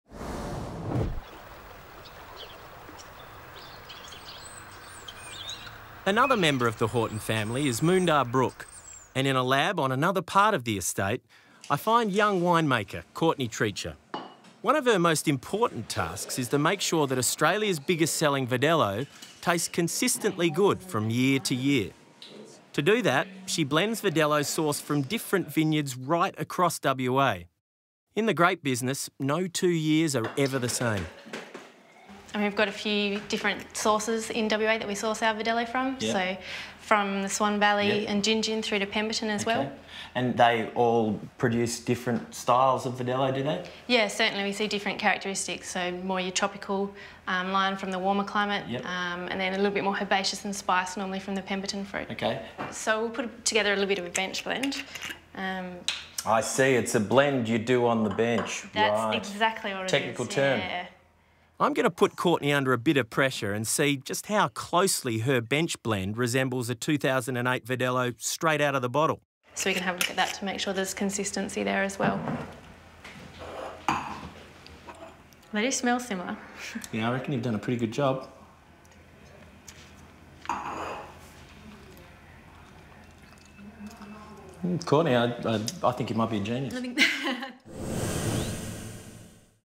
Stuart MacGill wines about Moondah Brook
Moondah Brook has long been a wine that is splashed about in the Australian summer.  Its well priced, easy on the uninitiated and always consistent... lets have a look at how they do it.